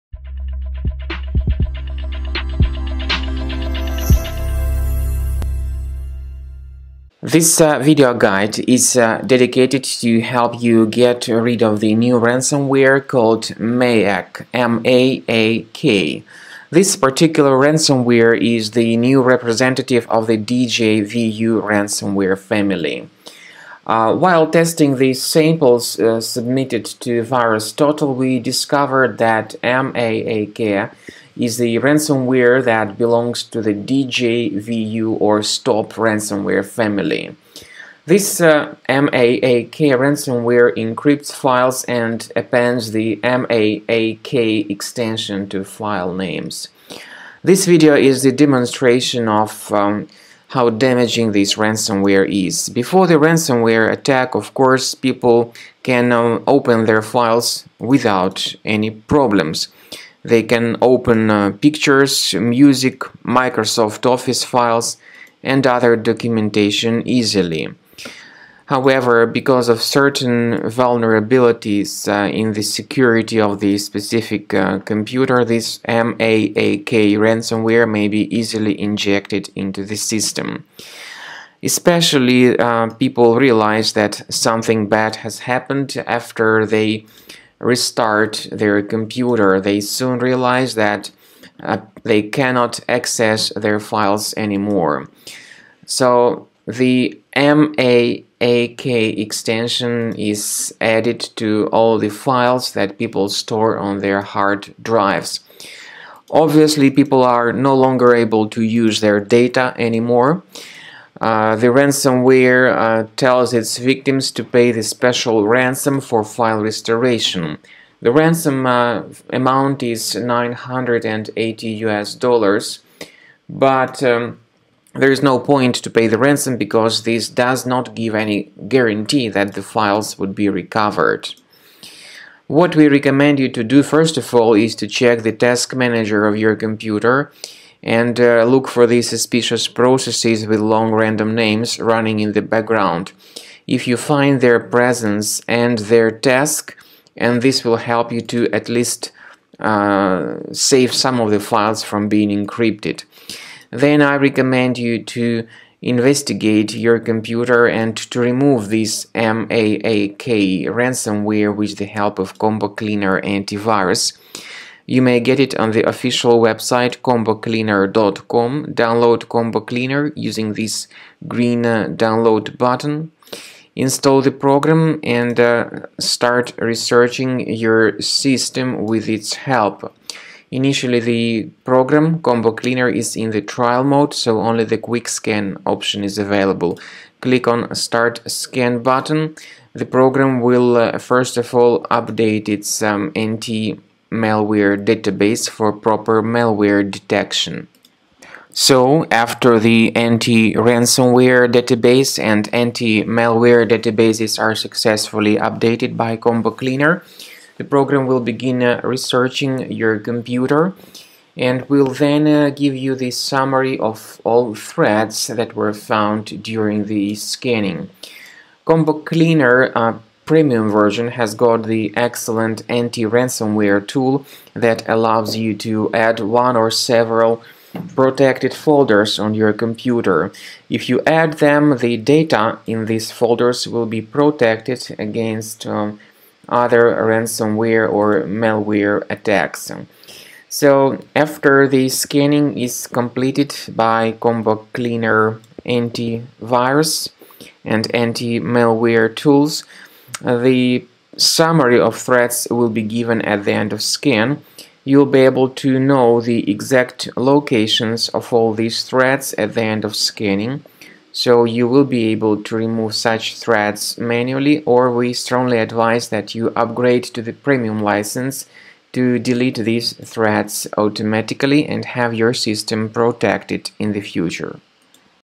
Remove Maak DJVU ransomware (Virus Removal Guide).
Maak is a new DJVU ransomware infection. We recommend Combo Cleaner Antivirus to protect your PC and files against it.

MAAK is a file-encrypting ransomware threat that blocks access to data (documents, pictures, videos) by encrypting files and ordaining them with the “.maak” extension. This tutorial instructs you how to delete MAAK ransomware by following simple step-by-step guidelines.

In case you can't open your pictures, documents, or files and they have got a .maak extension, then your PC is contaminated with the STOP/DJVU ransomware.
This ransomware encrypts the personal data stored on the victim’s PC with the “.maak” extension, then shows a message which instructs to decrypt the files if payment in Bitcoin is effected. The scary message is located on the victim’s desktop in the “_readme.txt” file.

However, remember that paying the ransom does not guarantee the file restoration, so you should not pay it!